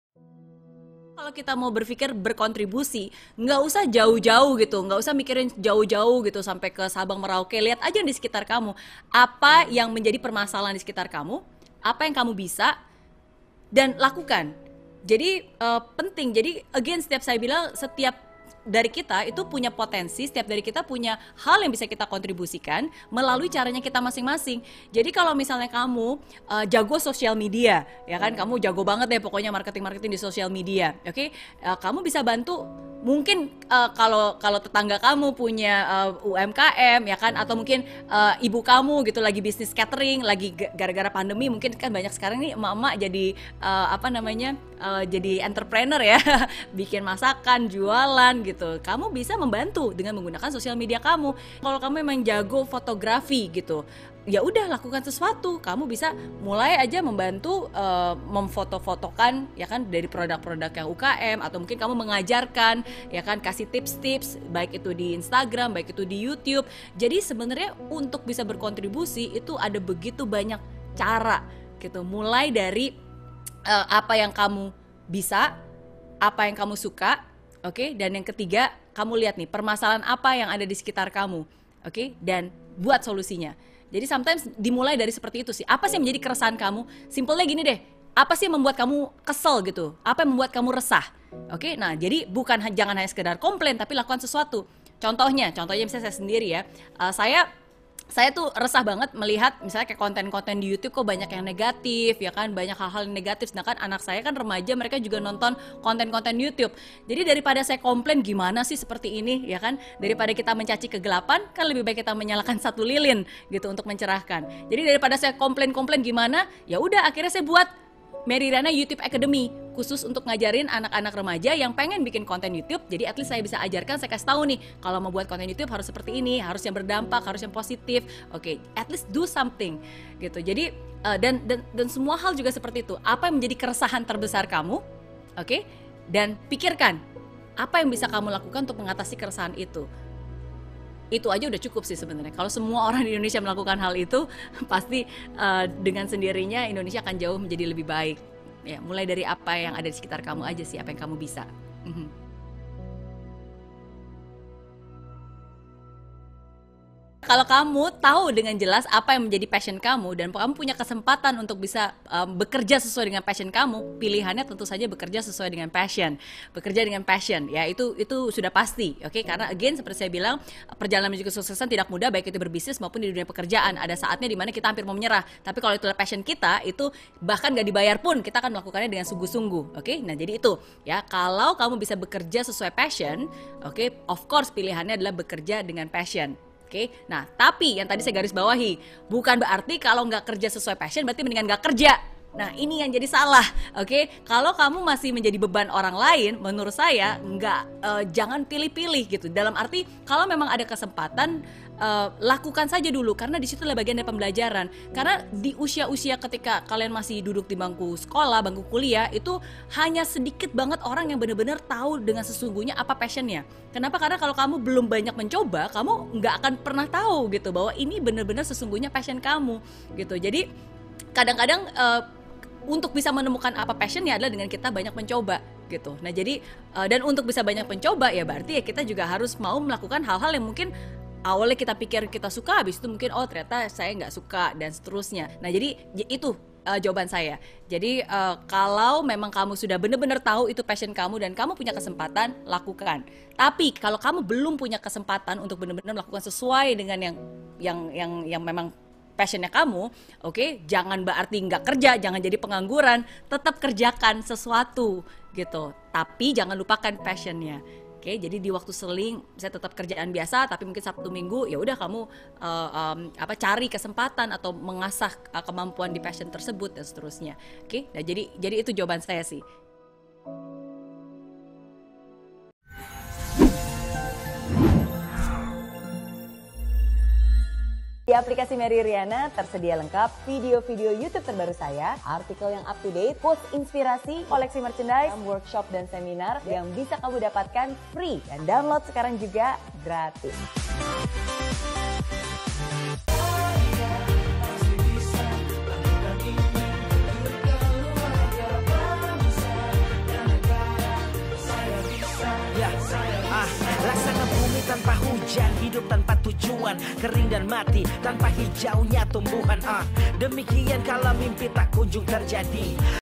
TONTON VIDEO INI KETIKA KAMU BINGUNG APA YANG MAU DILAKUKAN | Motivasi Merry | Merry Riana
Video Motivasi Merry Riana
Motivasi Merry | TONTON VIDEO INI KETIKA KAMU BINGUNG APA YANG MAU DILAKUKAN

Masih bingung apa yang mau kamu lakukan dalam hidup?
Semoga video berikut ini bisa menjadi jawaban atas kebingungan dan keresahan kamu selama ini ya :)

Semoga menginspirasi!